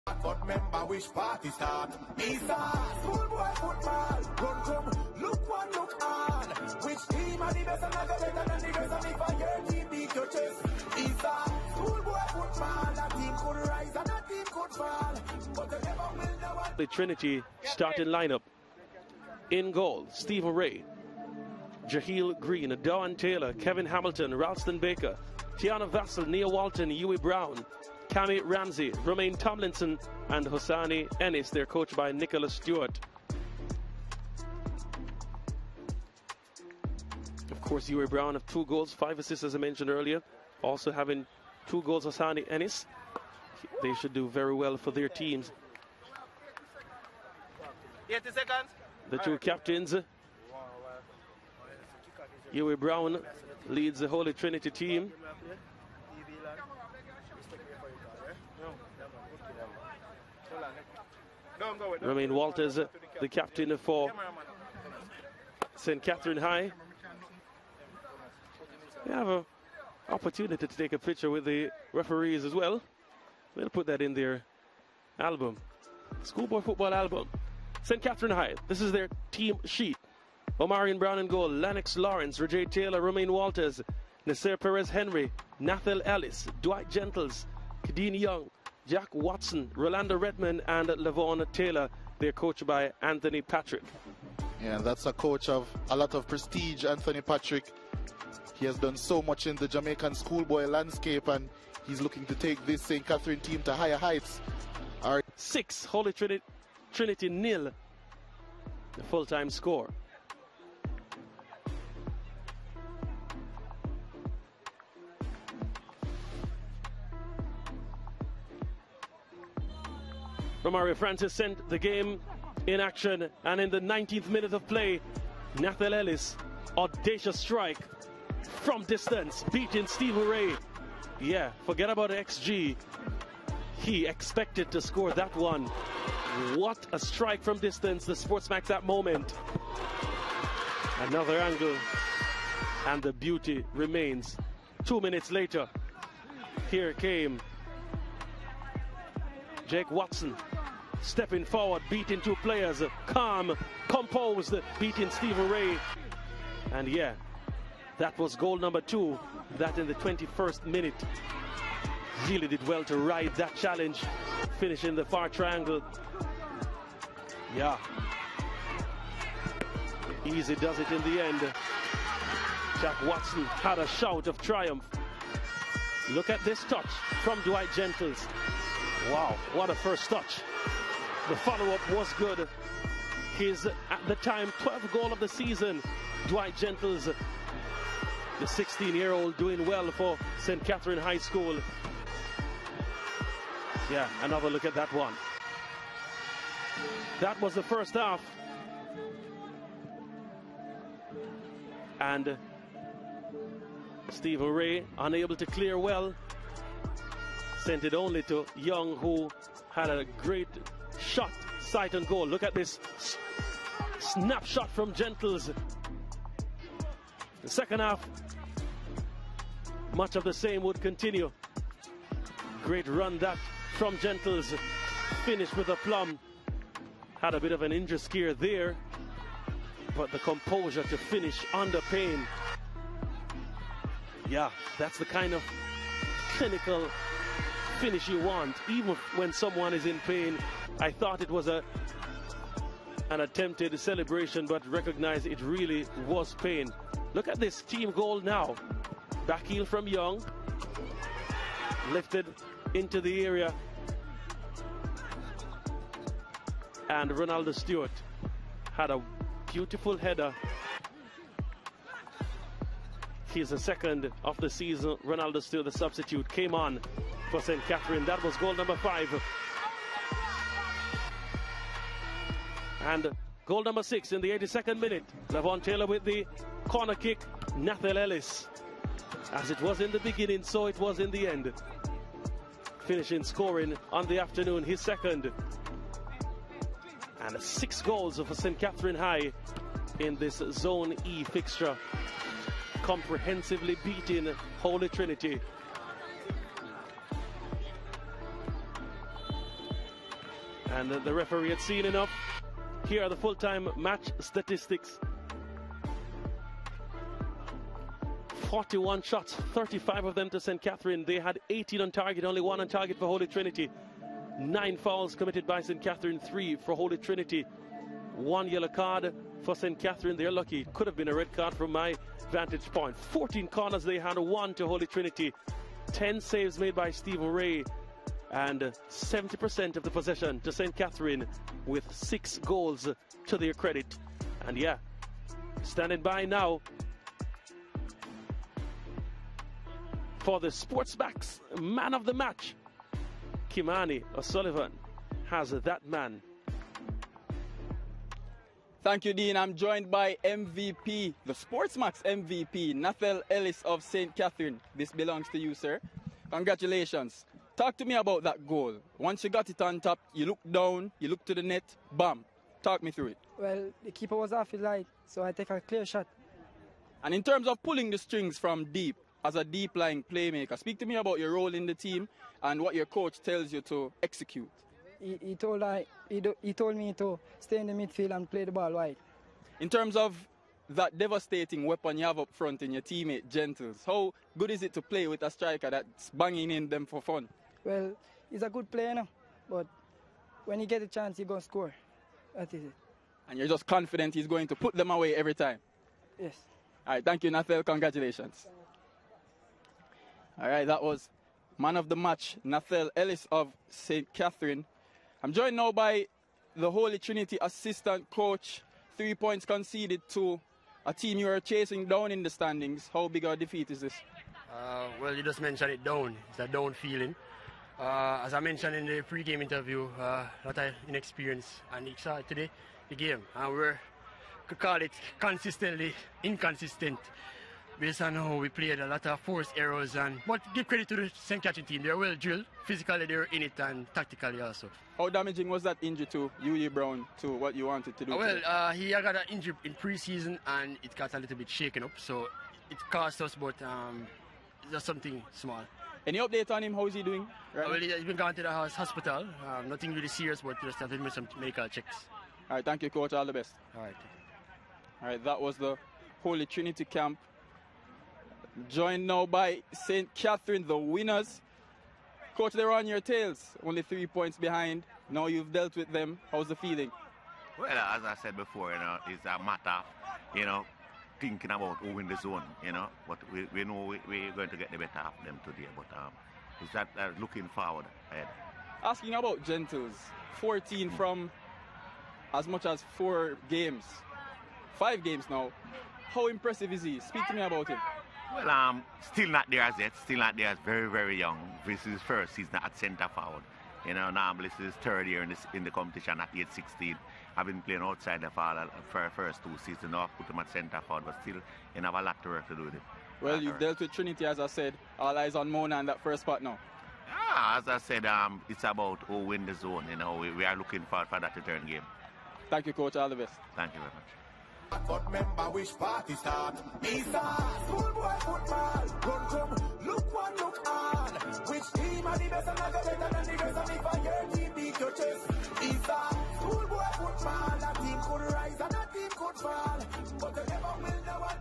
St. Catherine High THRASH Holy Trinity 6-0 in RD1 ISSA SBF Manning Cup contest! Match Highlights
SportsMax presents Round 1 Manning Cup highlights between Holy Trinity  High and St. Catherine High.